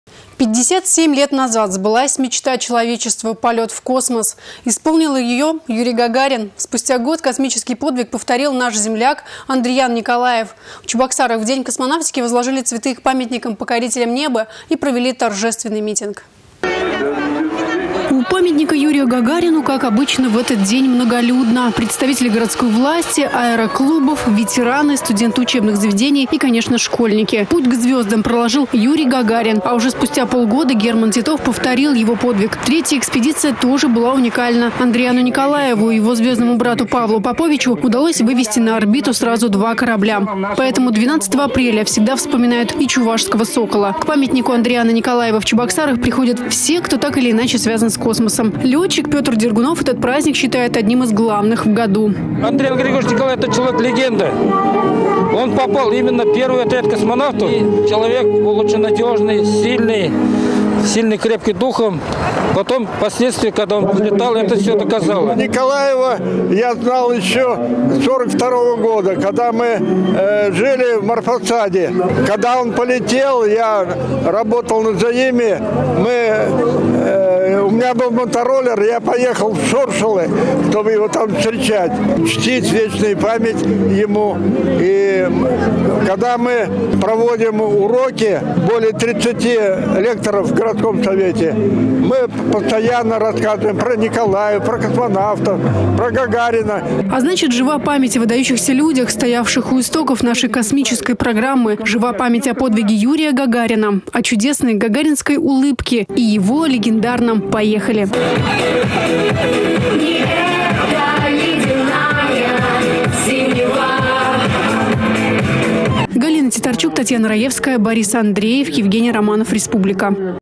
57 лет назад сбылась мечта человечества - полет в космос.
Исполнил ее Юрий Гагарин. Спустя год космический подвиг повторил наш земляк Андриян Николаев. В Чебоксарах в День космонавтики возложили цветы к памятникам покорителям неба и провели торжественный митинг.